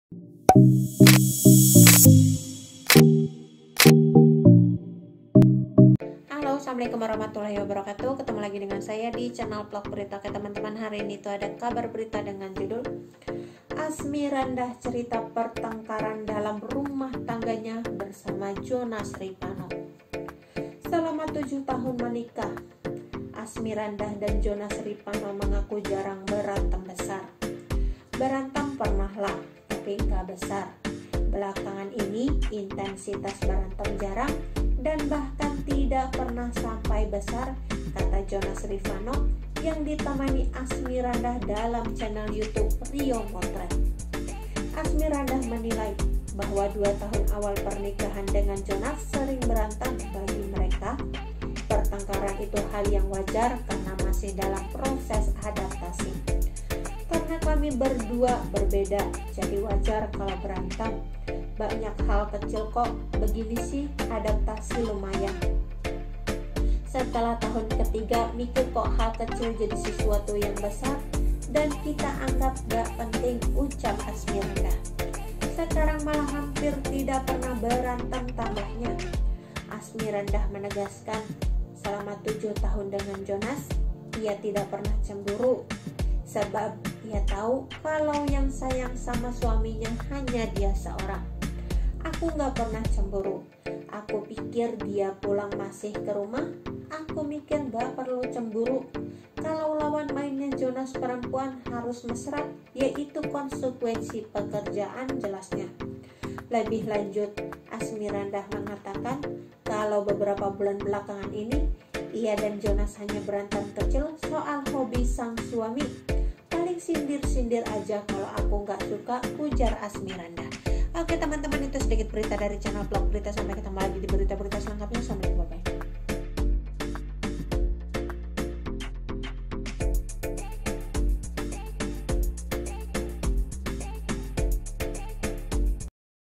Asmirandah Cerita Pertengkaran dalam Rumahtangganya Bersama Jonas Rivanno
Selama tujuh tahun menikah, Asmirandah (31) dan Jonas Rivanno mengaku jarang berantem besar.

"Berantem pernahlah tapi enggak besar. Belakangan ini intensitas berantem jarang dan bahkan tidak pernah sampai besar," kata Jonas Rivanno yang ditemani Asmirandah, dalam channel YouTube Rio Motret,
Asmirandah menilai bahwa dua tahun awal pernikahan dengan Jonas sering berantem. Bagi mereka, pertengkaran itu hal yang wajar karena masih dalam proses adaptasi.

"Karena kami berdua berbeda, jadi wajar kalau berantem. Banyak hal kecil kok begini sih, adaptasi lumayan. Setelah tahun ketiga mikir kok hal kecil jadi sesuatu yang besar dan kita anggap gak penting," ucap Asmirandah.
"Sekarang malah hampir tidak pernah berantem," tambahnya.

Asmirandah menegaskan selama menikah dengan Jonas, ia tidak pernah cemburu. Sebab, ia tau kalau yang sayang sama suaminya hanyalah dia seorang.

"Aku enggak pernah cemburu. Aku mikir dia (Jonas) pulang masih ke rumah. Aku mikir enggak perlu cemburu. Kalau lawan mainnya Jonas perempuan harus mesra